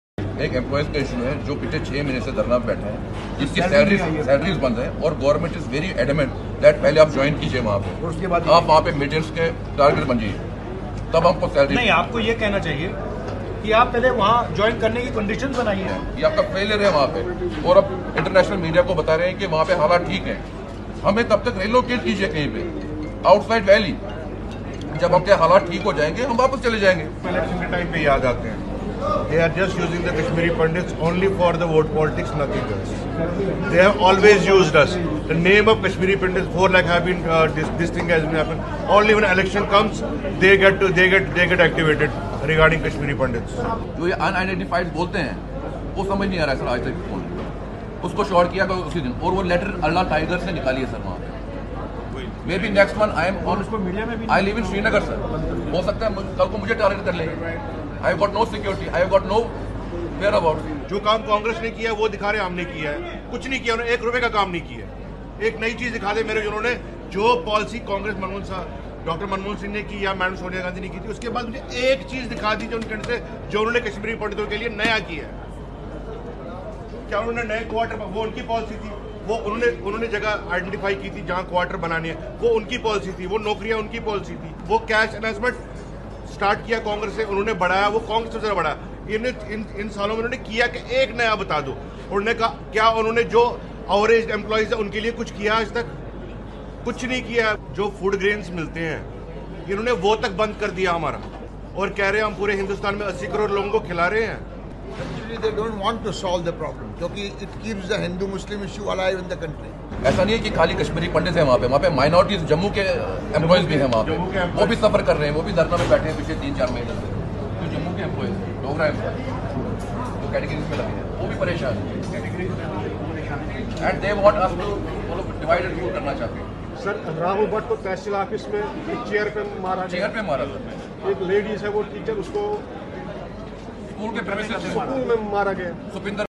जम्मू-कश्मीर में राहुल गांधी से प्रवासी कश्मीरी पंडितों के प्रतिनिधिमंडल से मुलाकात | ASD News
Bharat Jodo Yatra: राहुल गांधी की भारत जोड़ो यात्रा इन दिनों जम्मू-कश्मीर में हैं।  सोमवार को प्रवासी कश्मीरी पंडितों प्रतिनिधिमंडल ने भारत जोड़ो यात्रा के दौरान राहुल गांधी से मुलाकात की। कश्मीरी पंडितों ने राहुल गांधी से की शिकायत। पंडितों ने राहुल से कहा 'BJP हमारा इस्तेमाल सिर्फ वोट की राजनीति के लिए करती है'.